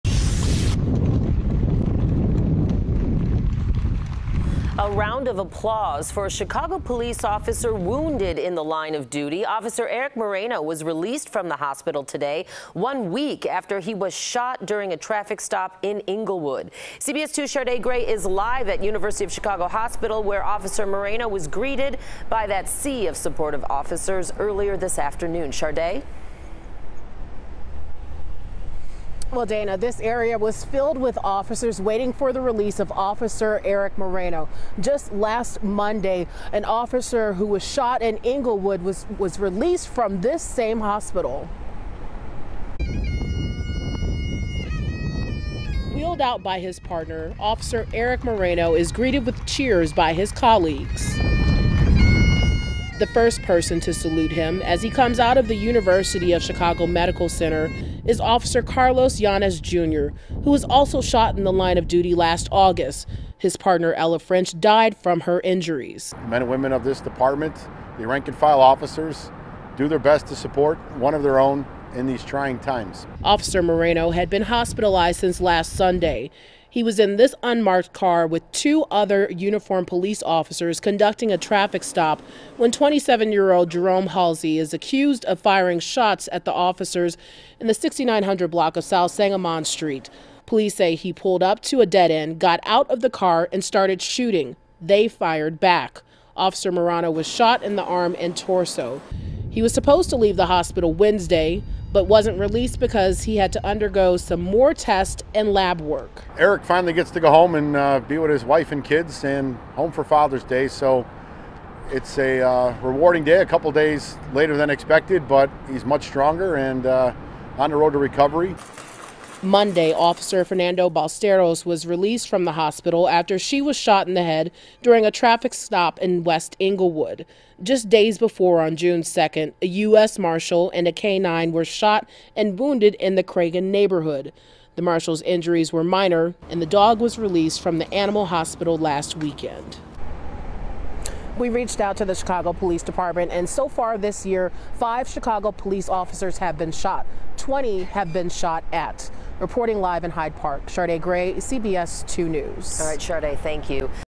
Officer wounded in Englewood released from hospital one week after shooting
Chicago Police officer Erik Moreno has been released from the hospital after he was shot during an attempted traffic stop in Englewood last weekend.